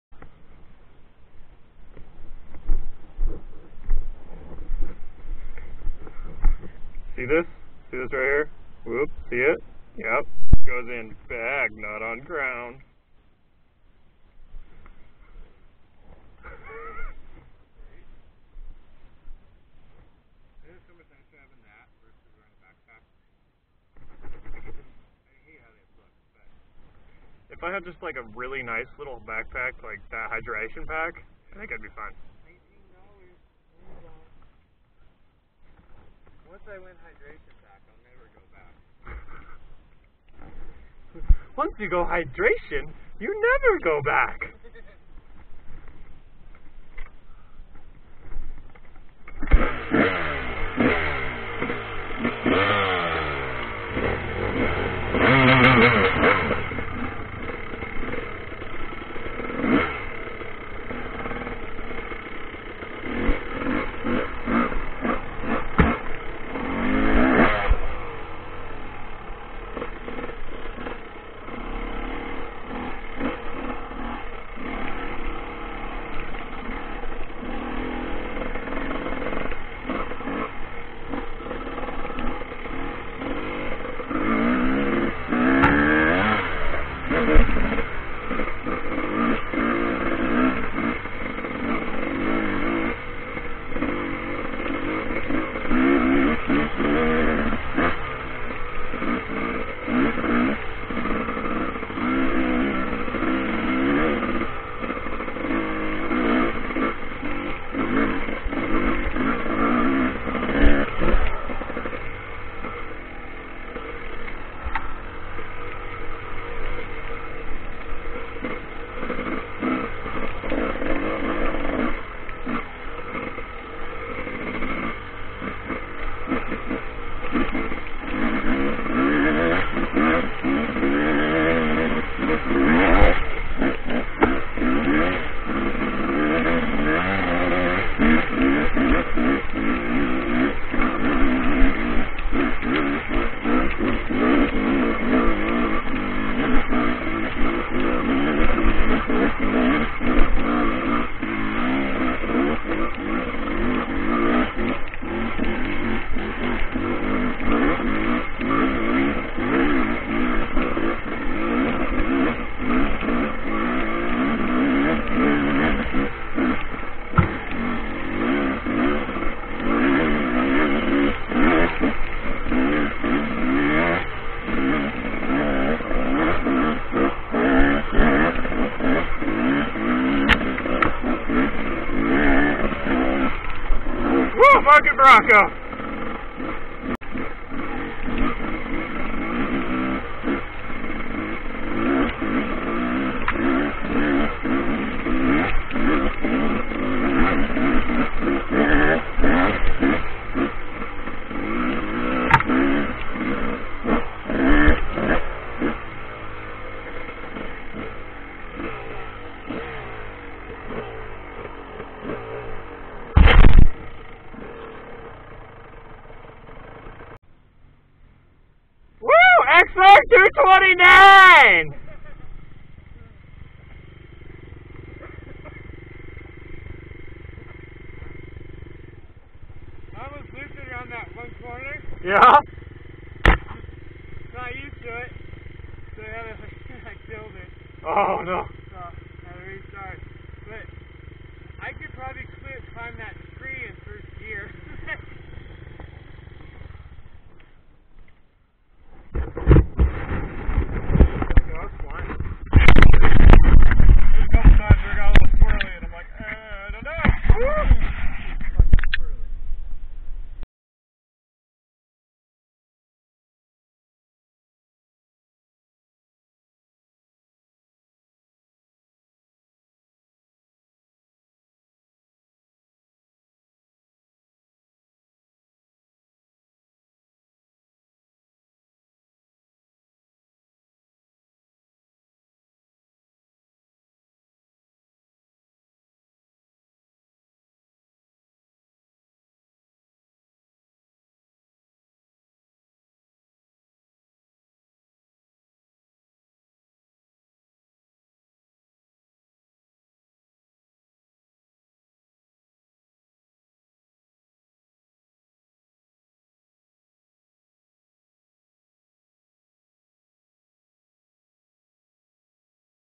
A little Hill climb.
Running the Cr250R and Xr229R up a hill.

Pack it in Pack it out don't throw your trash on the ground